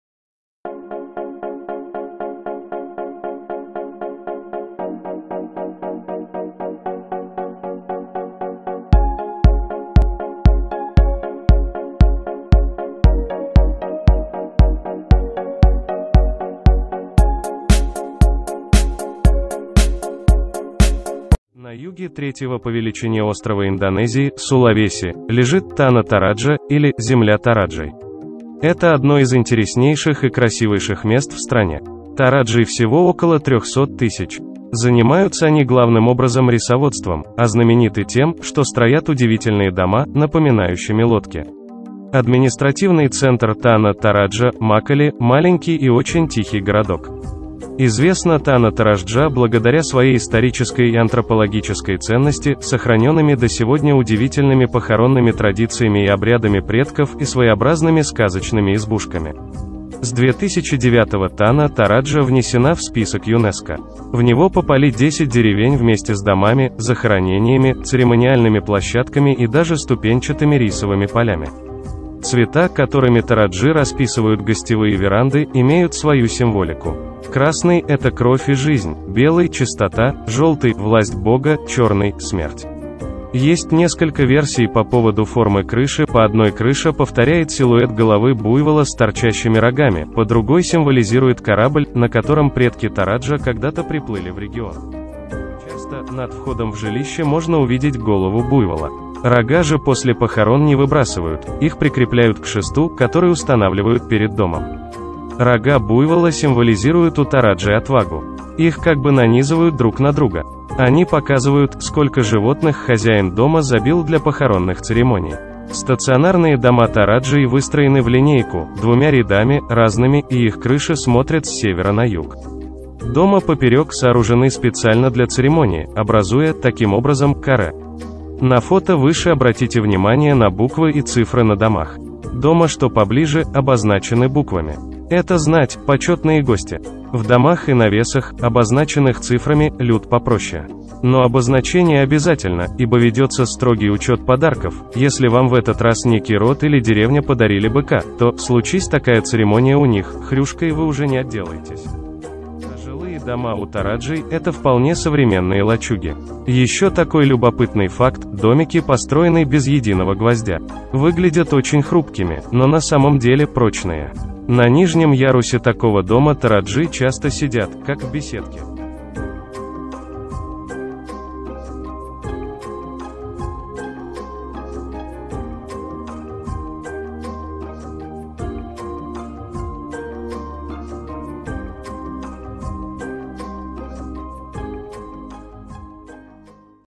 ТРАДИЦИОННЫЕ ДОМА ТАНА ТОРАДЖА. СУЛАВЕСИ. ИНДОНЕЗИЯ.
На юге третьего по величине острова Индонезии, Сулавеси, лежит «Тана Тораджа», или «земля тораджей». Это одно из интереснейших и красивейших мест в стране. Тораджей всего около 300 тысяч. Занимаются они главным образом рисоводством, а знамениты тем, что строят удивительные дома, напоминающими лодки. Административный центр Тана Тораджа — Макале, маленький и очень тихий городок.
Известна Тана Тораджа благодаря своей исторической и антропологической ценности — сохраненными до сегодня удивительными похоронными традициями и обрядами предков и своеобразными сказочными избушками.

С 2009-го Тана Тораджа внесена в список ЮНЕСКО. В него попали 10 деревень вместе с домами, захоронениями, церемониальными площадками и даже ступенчатыми рисовыми полями.

Цвета, которыми тораджи расписывают гостевые веранды, имеют свою символику. Красный — это кровь и жизнь, белый - чистота, желтый - власть Бога, черный - смерть.

Есть несколько версий по поводу формы крыши – по одной крыша повторяет силуэт головы буйвола с торчащими рогами, по другой символизирует корабль, на котором предки тораджа когда-то приплыли в регион.